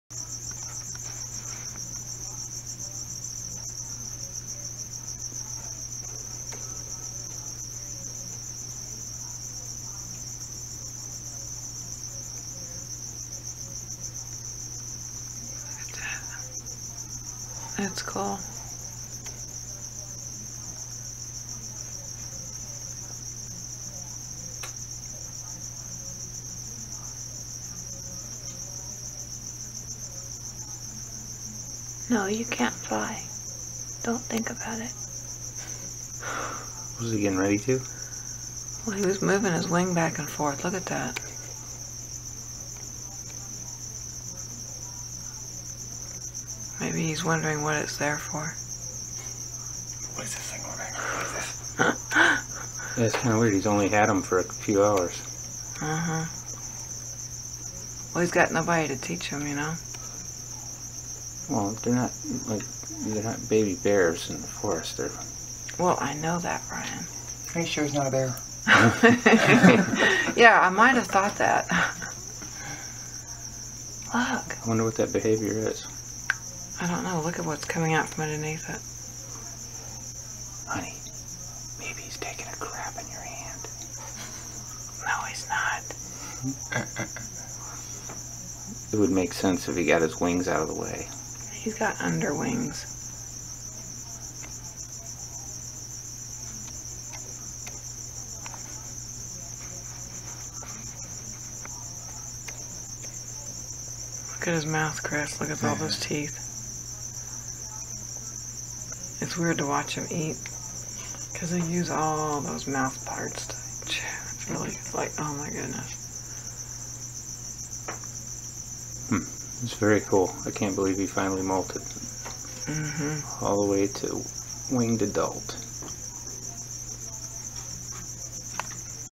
wahlbergi adultmolt 101414
We had a winged adult wahlbergi this morning.  Very exciting!